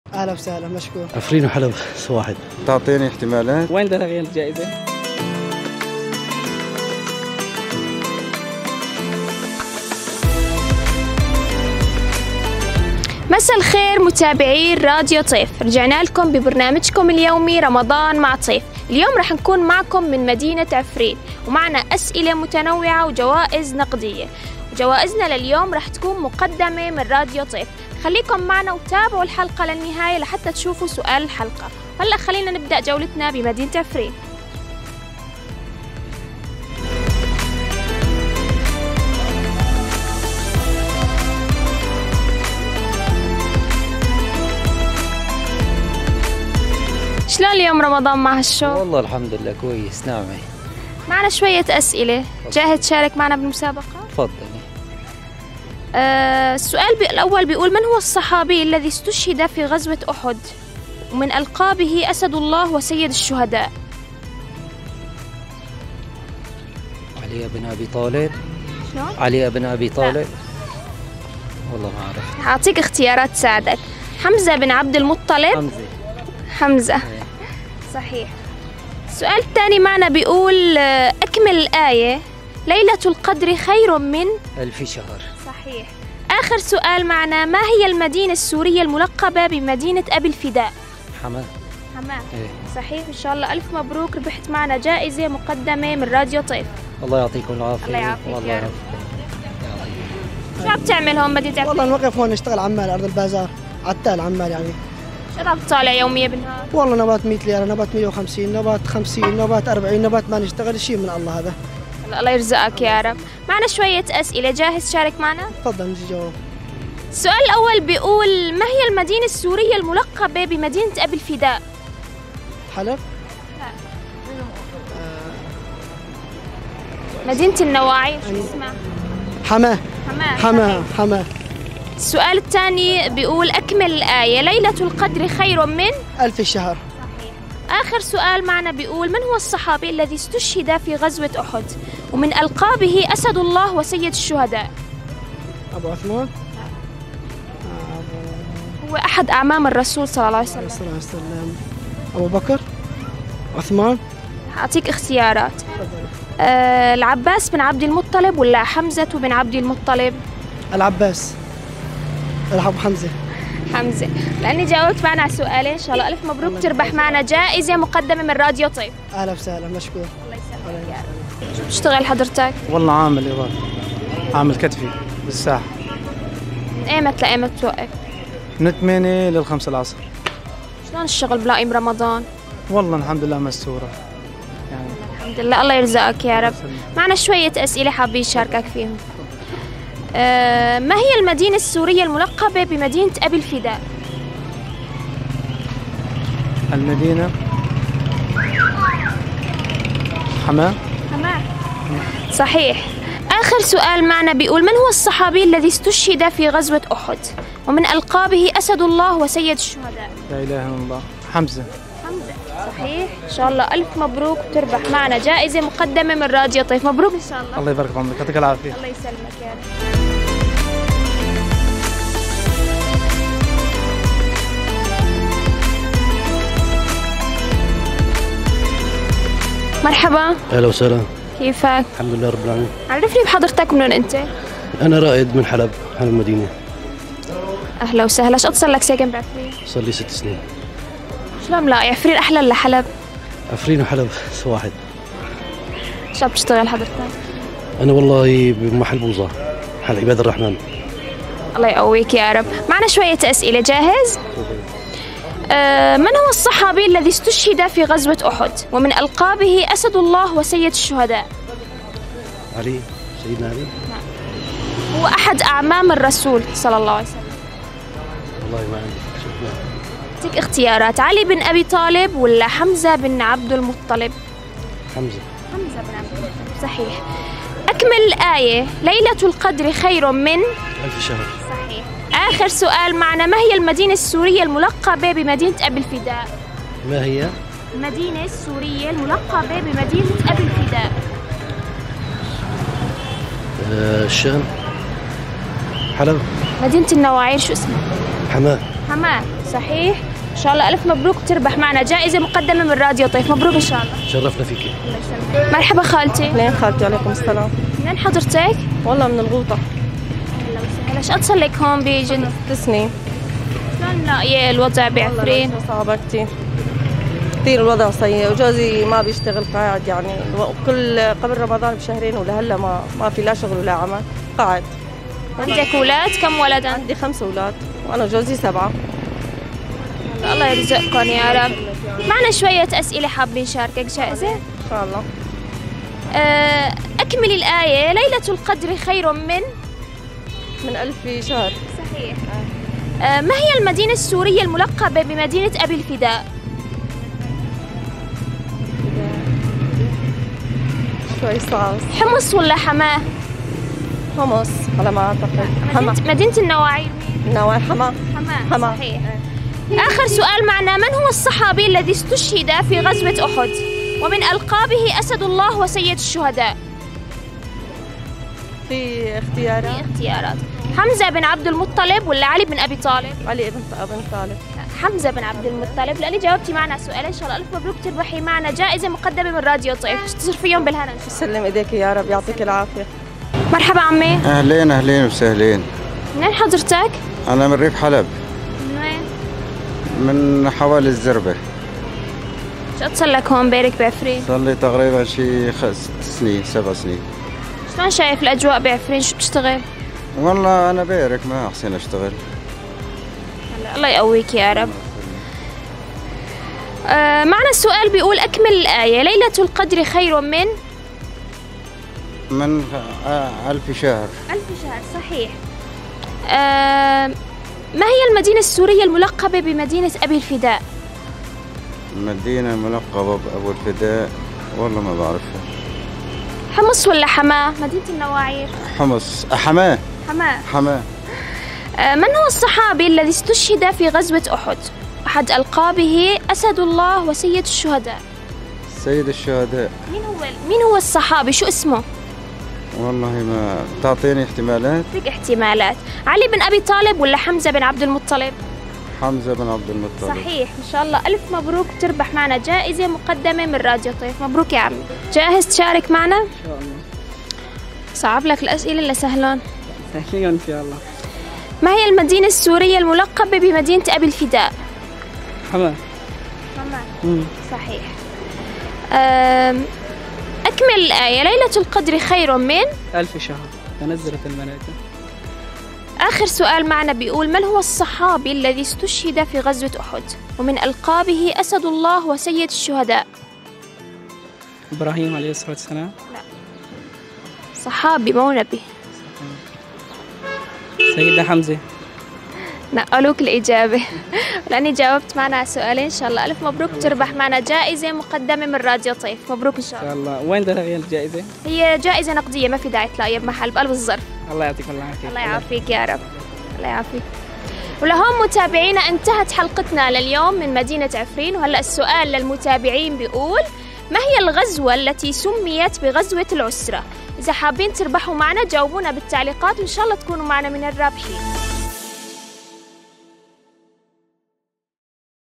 حلقة اليوم من مدينة عفرين زينة ريف حلب بجمالها وطبيعتها الساحرة، تابعوا حلقة رمضان مع طيف ولا تنسو
حلقة اليوم من مدينة عفرين زينة ريف حلب بجمالها وطبيعتها الساحرة، تابعوا حلقة رمضان مع طيف ولا تنسوا الإجابة على سؤال المتابعين ضمن التعليقات للمشاركة بالسحب على جائزة نقدية.
ماهي الغزوة التي سميت بغزوة العُسرة؟